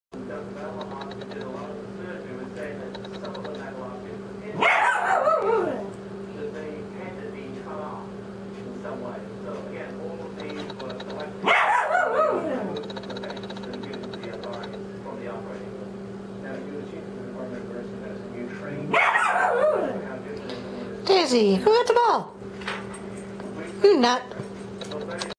Daisy barking at her squeaky ball
Daisy does this a lot.  I have no idea what she's thinking but it seems to be fun for her.